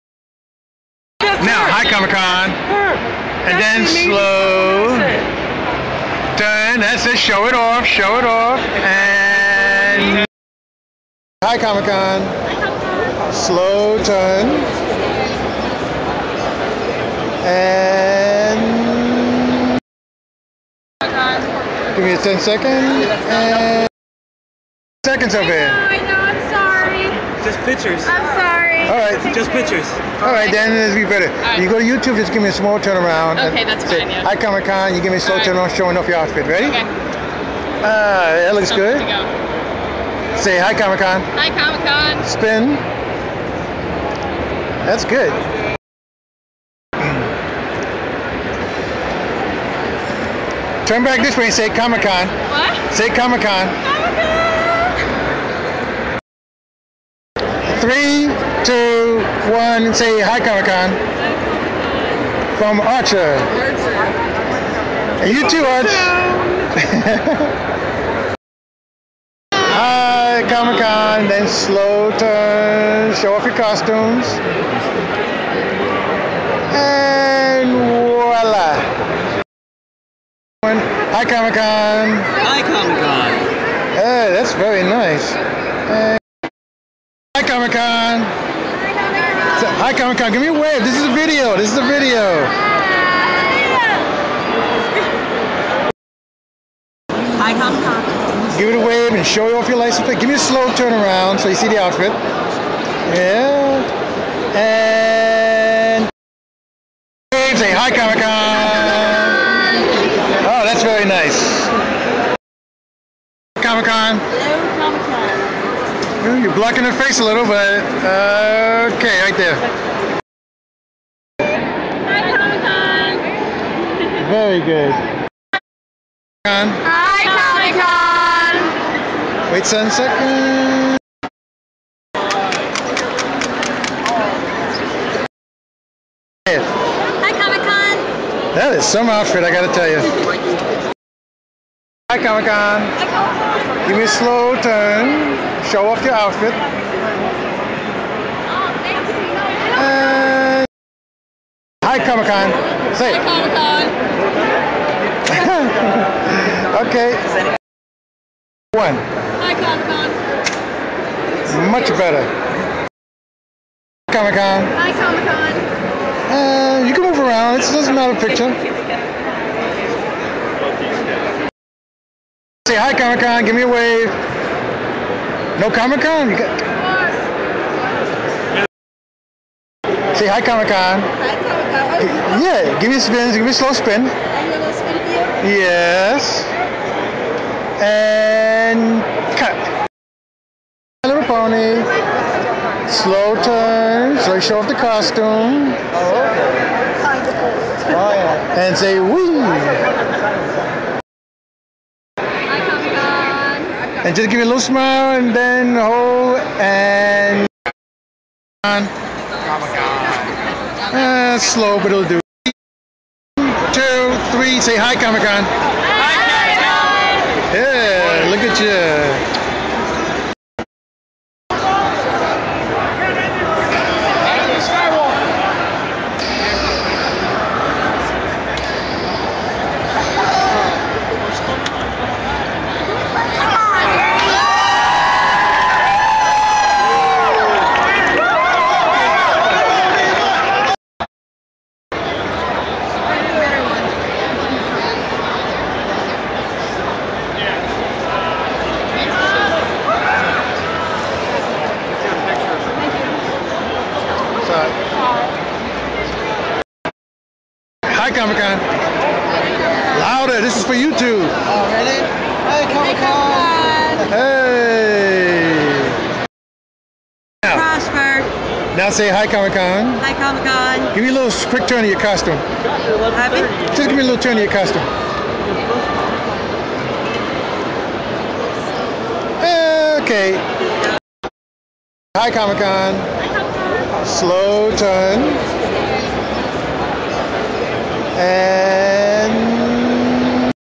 COMIC-CON 2012 (HELLO COMIC-CON) PART-1
Sorry everybody new camara will do better next year, will brake this down in several parts.My first time there, love the costumes pitty no one dress like Selene form UNDERWORLD hope to see some next year.